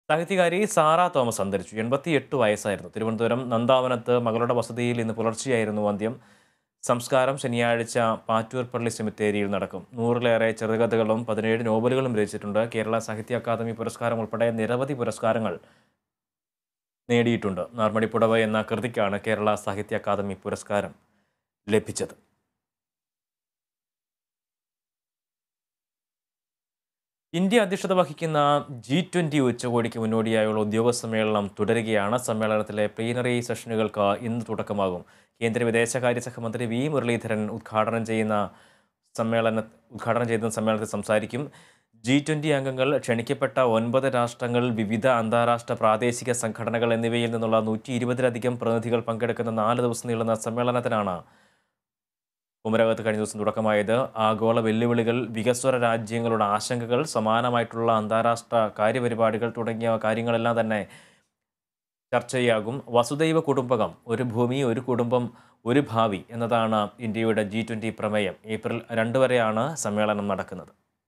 സാഹിത്യകാരി സാറാ തോമസ് അന്തരിച്ചു | Sarah Thomas
സാഹിത്യകാരി സാറാ തോമസ് അന്തരിച്ചു. 88 വയസ്സായിരുന്നു. തിരുവനന്തപുരം നന്ദാവനത്ത് മകളുടെ വസതിയില്‍ ഇന്ന് പുലര്‍ച്ചെയായിരുന്നു അന്ത്യം. സംസ്‌കാരം ശനിയാഴ്ച പാറ്റൂര്‍ പള്ളി സെമിത്തേരിയില്‍ നടക്കും. നൂറിലേറെ ചെറുകഥകളും 17 നോവലുകളും രചിചിട്ടുണ്ട്.  കേരള സാഹിത്യ അക്കാദമി പുരസ്‌കാരമുള്‍പ്പെടെ നിരവധി പുരസ്‌കാരങ്ങള്‍ നേടിയിട്ടുണ്ട്. നാര്‍മടിപ്പുടവ എന്ന കൃതിക്കാണ് കേരള സാഹിത്യ അക്കാദമി പുരസ്‌കാരം ലഭിച്ചത്.

About COA